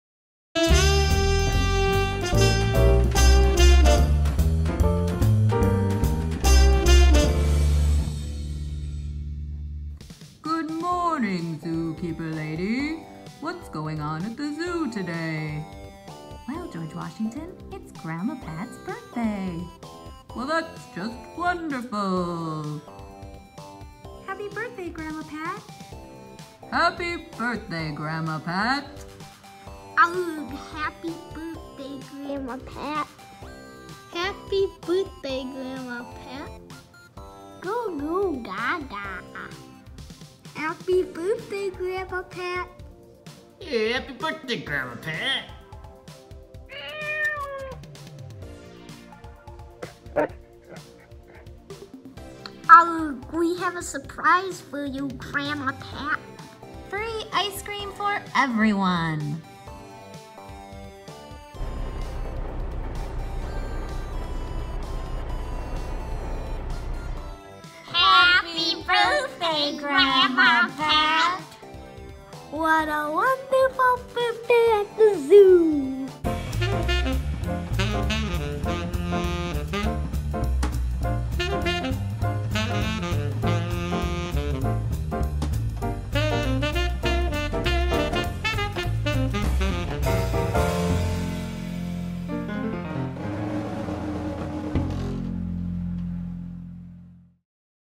Gramma Pat's Birthday at the Zoo
What a special day at the Playmobil Zoo!  Time for a birthday party!